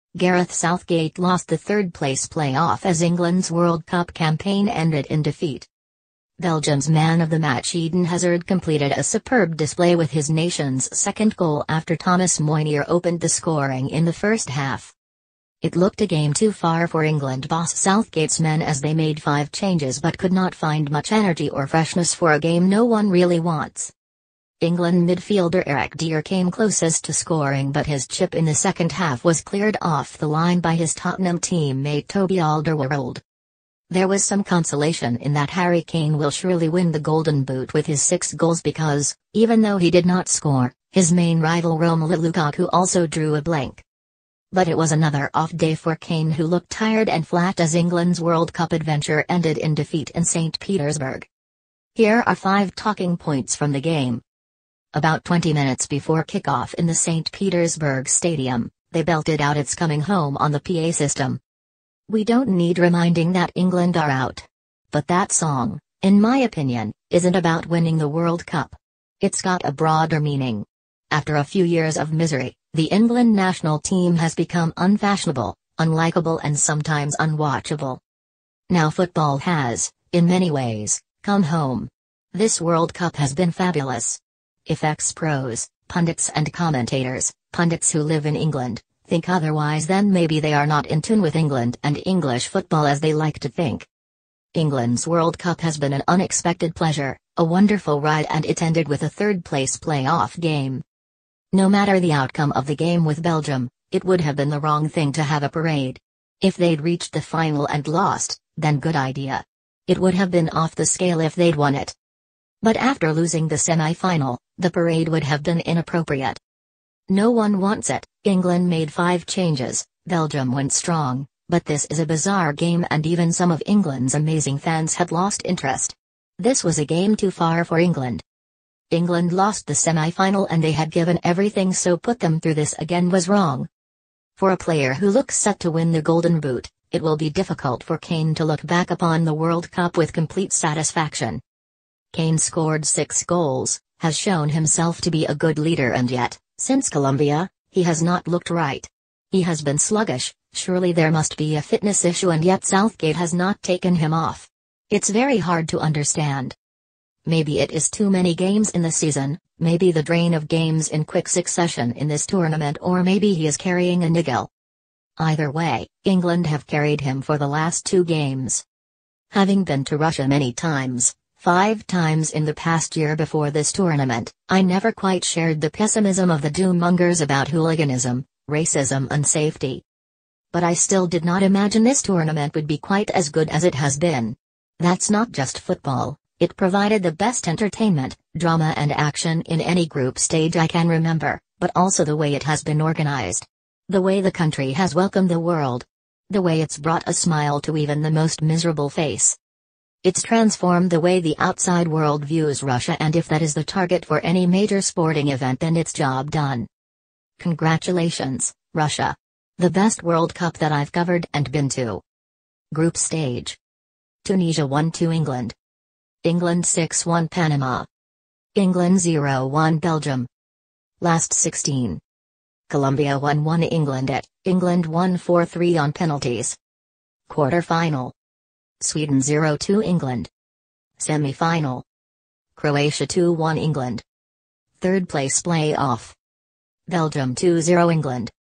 Belgium 2-0 England: Thomas Meunier and Eden Hazard win World Cup 2018 third-place play-off - 5 talk
Gareth Southgate lost the third place play-off as England’s World Cup campaign ended in defeat.
Belgium’s man of the match Eden Hazard completed a superb display with his nation's second goal after Thomas Meunier opened the scoring in the first half.